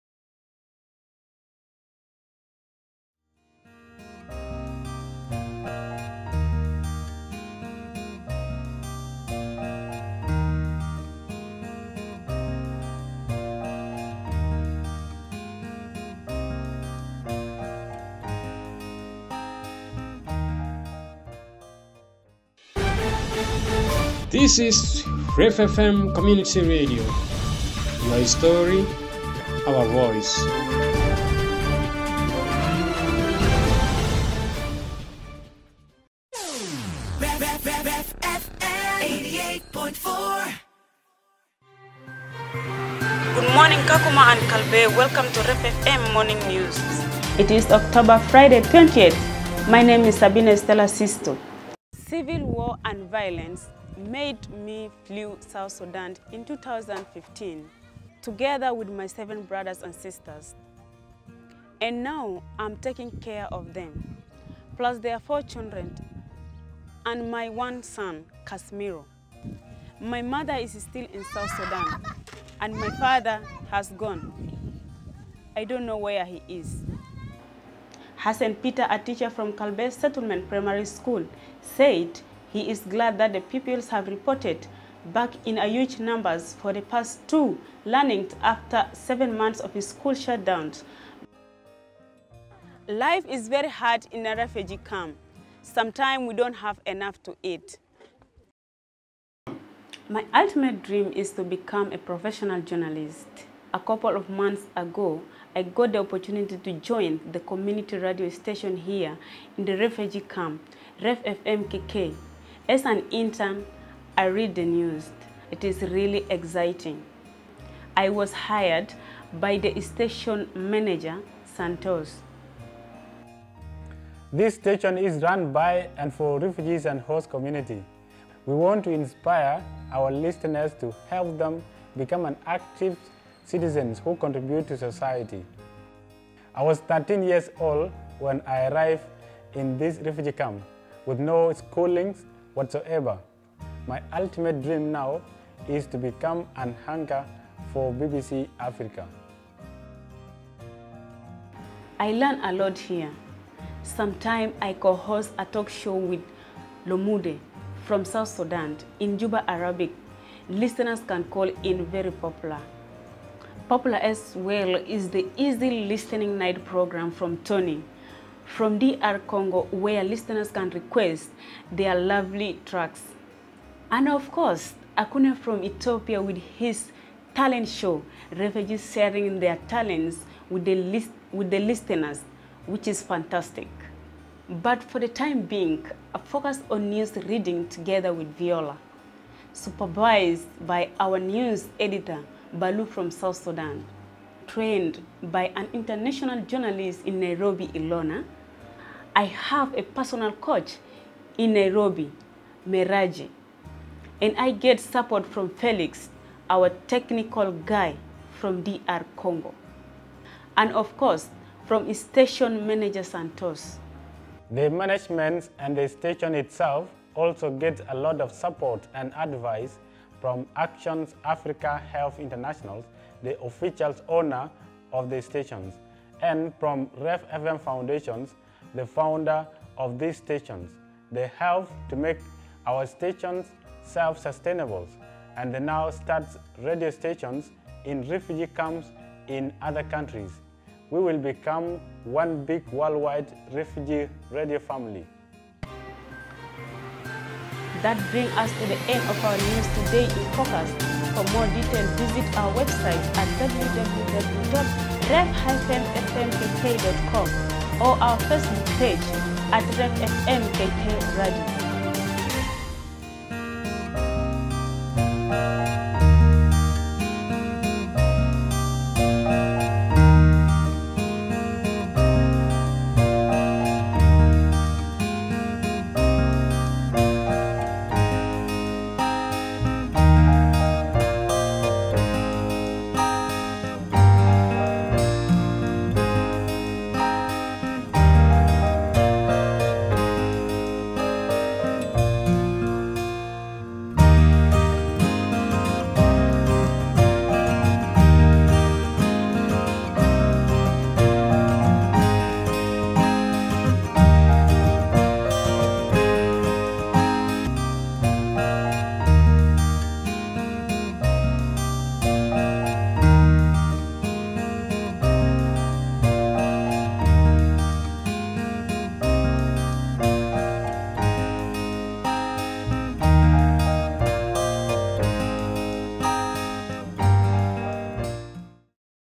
Radio station for/by refugees
Stella tells the story of the first radio station in a refugee camp initiated by REF FM Foundation: REF FM KK in Kakuma-Kalobeyei in Kenya,

Interested to start your own radio station in a refugee camp, to inspire refugees (and their hosts) and to give them a voice and? Contact And listen to Stella and her colleagues from the station in Kenya